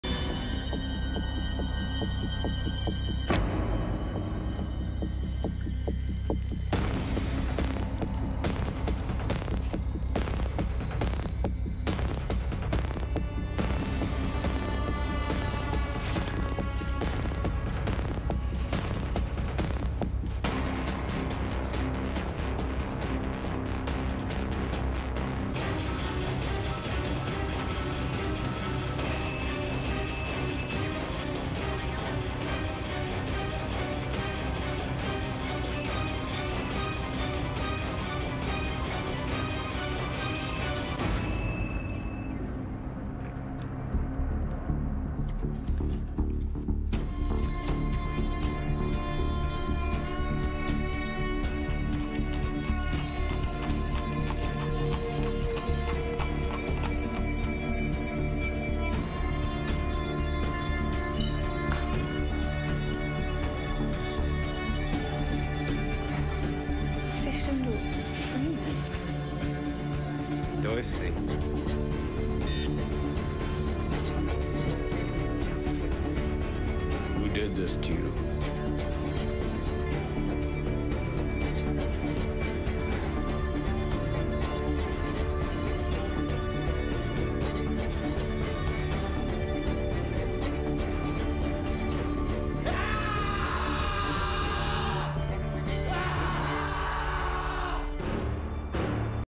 Freeman Speaks, Briefly
Freeman comes to see Esther and speaks for the first time.